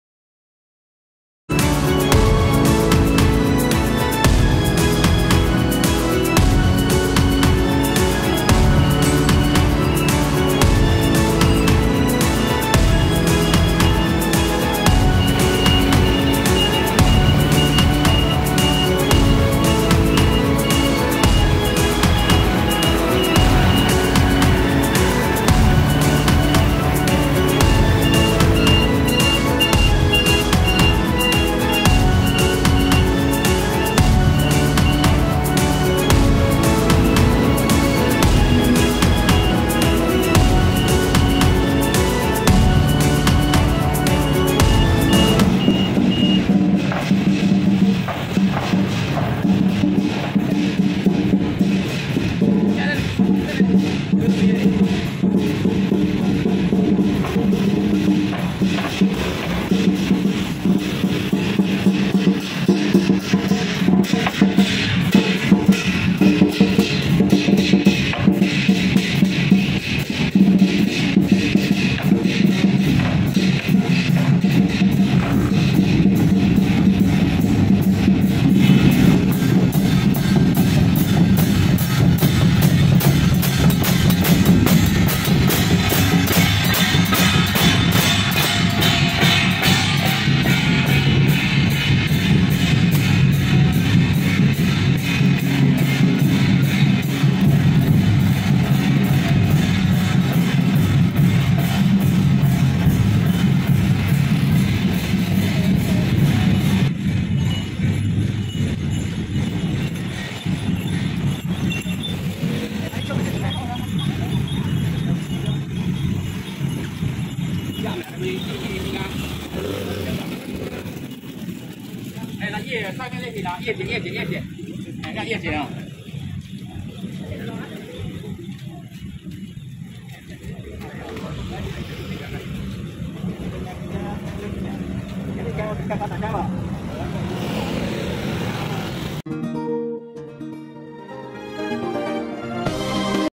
振王宮會香回石榴
拍攝日：5/28
振王宮南下會香之回石榴